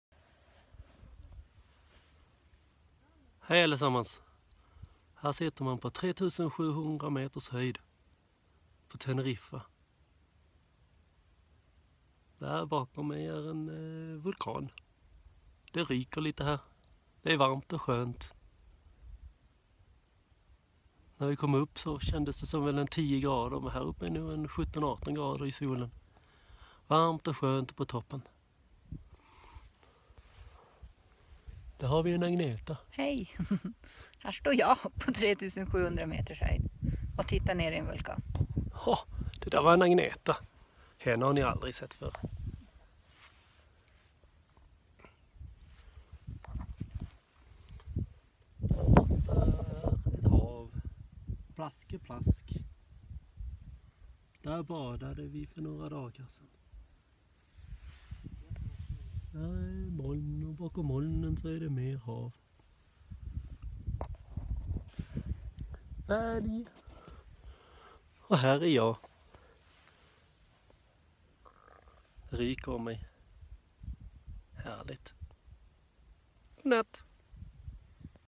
Hej från toppen av Teijde.
En liten pratstund med den blåa himlen på 3718m höjd.